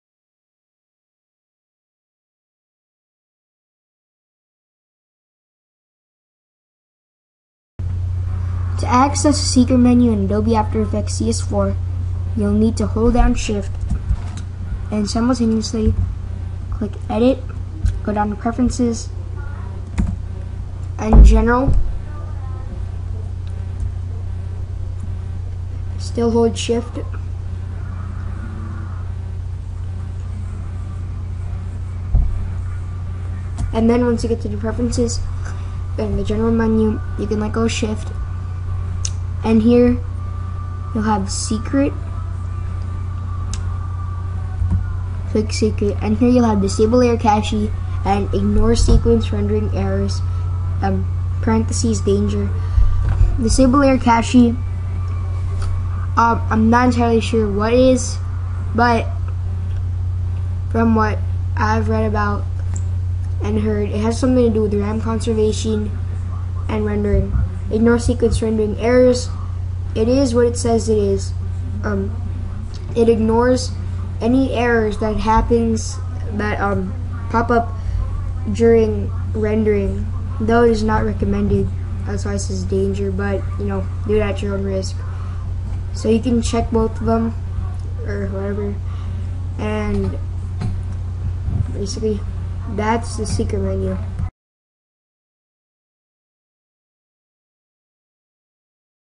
How to Access the Secret Menu in After Effects
This is a tutorial on how to access the Secret Menu in Adobe After Effects CS4.